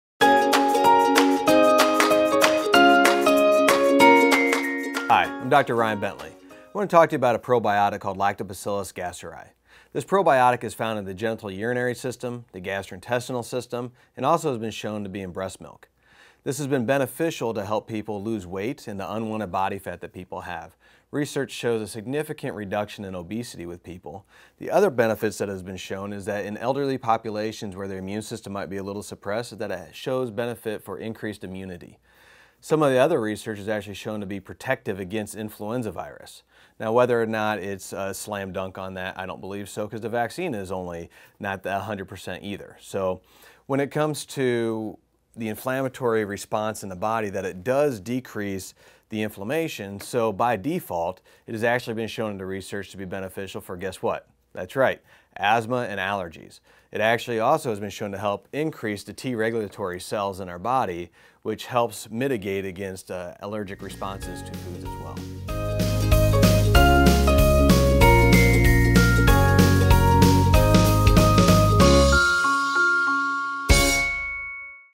Lactobacillus Gasseri
TRANSCRIPT
Hi, I'm Dr. Ryan Bentley. I want to talk to you about a probiotic called lactobacillus gasseri. This probiotic is found in the genital urinary system, the gastrointestinal system, and also has been shown to be in breast milk. This has been beneficial to help people lose weight and the unwanted body fat that people have. Research shows a significant reduction in obesity with people. The other benefits that has been shown is that in elderly populations where their immune system might be a little suppressed, that it shows benefit for increased immunity. Some of the other research has actually shown to be protective against influenza virus. Now, whether or not it's a slam dunk on that, I don't believe so because the vaccine is only not that 100% either. So, when it comes to the inflammatory response in the body, that it does decrease the inflammation. So, by default, it has actually been shown in the research to be beneficial for guess what? That's right, asthma and allergies. It actually also has been shown to help increase the T regulatory cells in our body, which helps mitigate against allergic responses to foods as well.